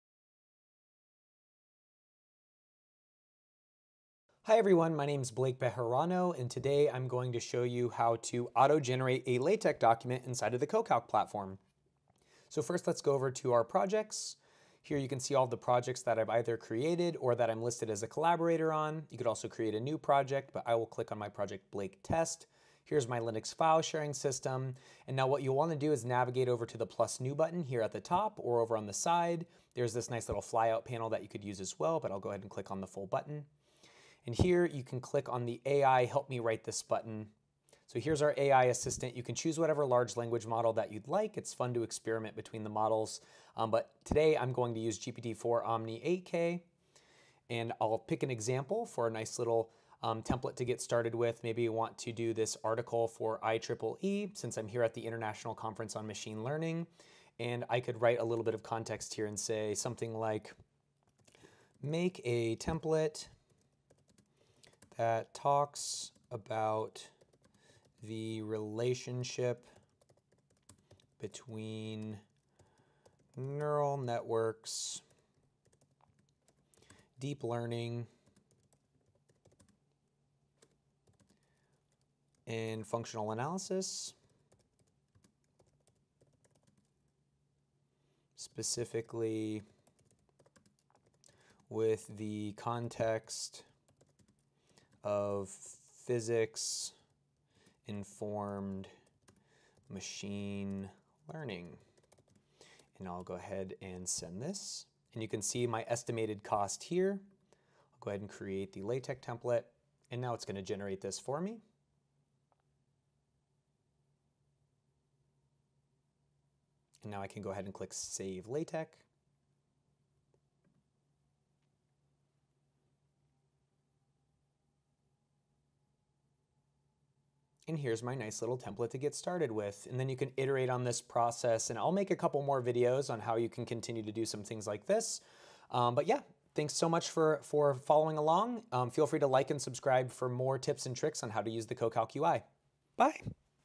How to AI Generate LaTeX in CoCalc
In this video, we’ll explore how to use AI tools in CoCalc to generate LaTeX documents. You’ll learn how to:

- Access AI features within CoCalc.
- Input prompts for generating LaTeX code.
- Edit and refine the generated content in your LaTeX document.

Using AI for LaTeX is great for:

- Quickly producing equations and documents.
- Enhancing productivity by automating formatting tasks.
- Experimenting with different document structures and styles.

Join us and discover how AI can streamline your LaTeX workflow in CoCalc!